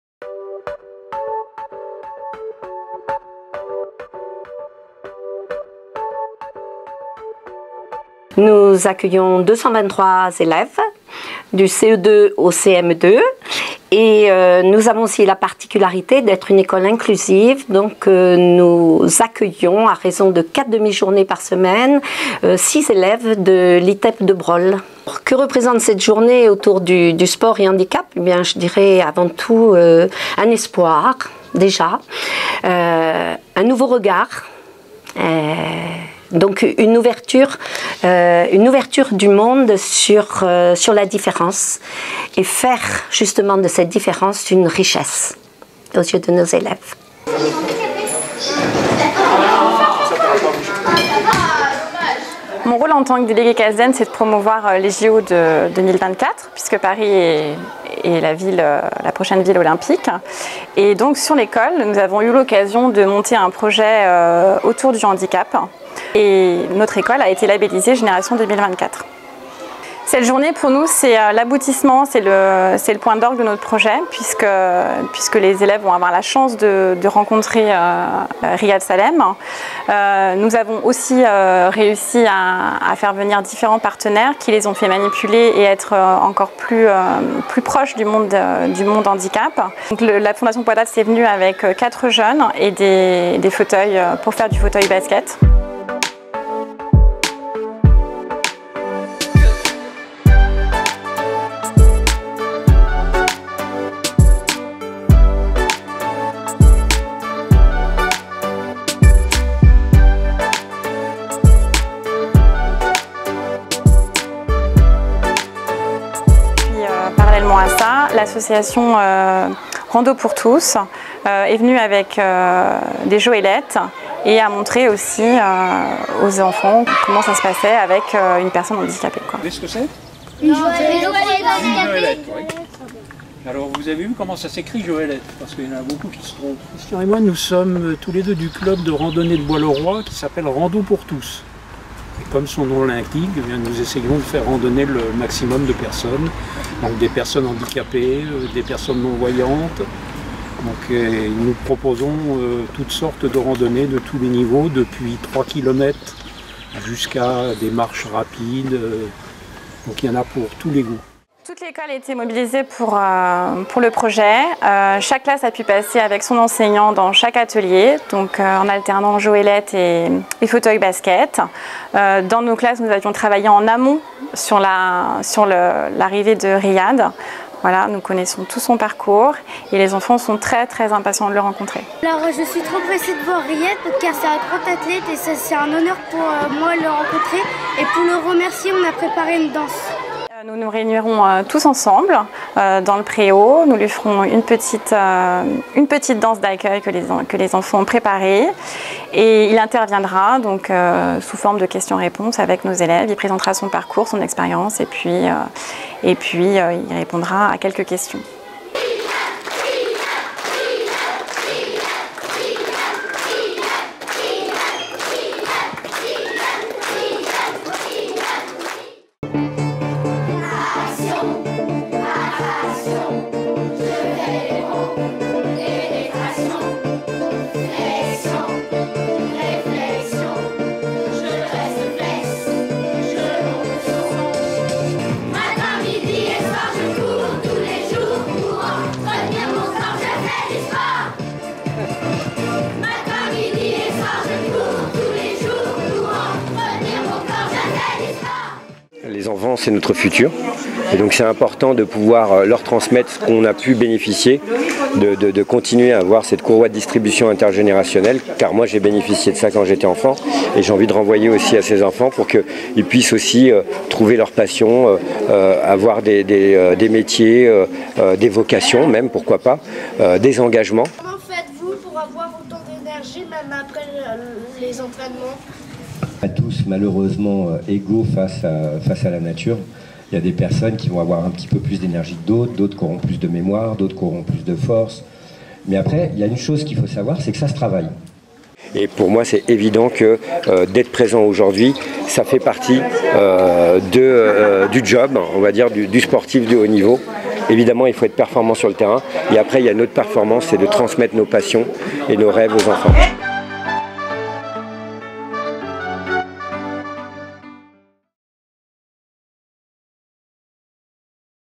Une journée sport et handicap, dans une école labellisée Génération 2024
L'école Olivier Métra, à Bois-le-Roi (Seine-et-Marne) est labellisée Génération 2024, suite à son engagement dans un projet d'école autour du sport et du handicap. Pour fêter la Journée Olympique, elle a organisé une journée d'activités le 25 juin, en présence du grand champion paralympique Ryadh Sallem.